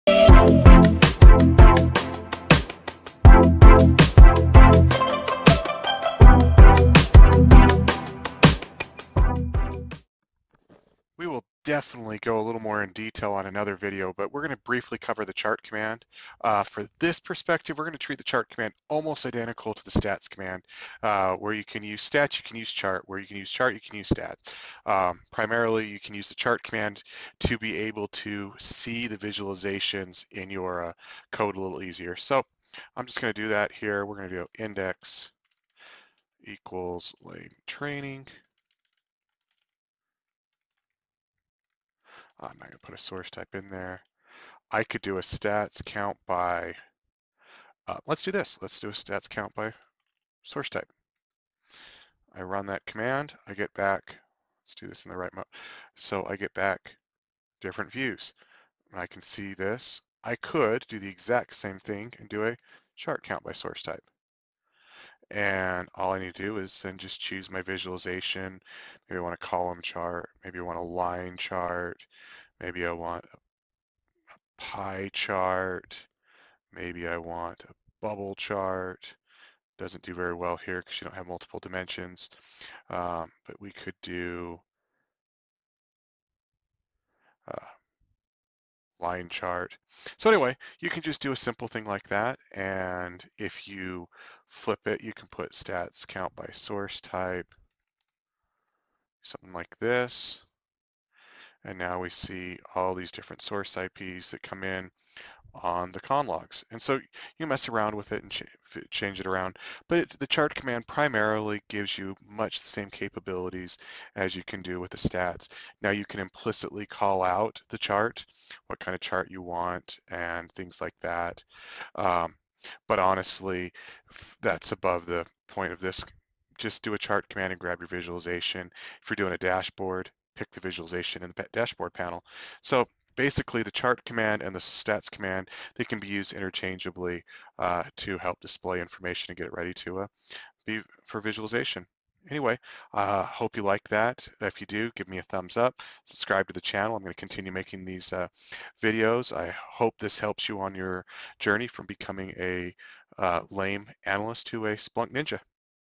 Splunk Chart Command Tutorial
Splunk tutorial on how to use the chart command in an SPL query.

Visit our discord channel to post questions and suggestions for what you want to learn.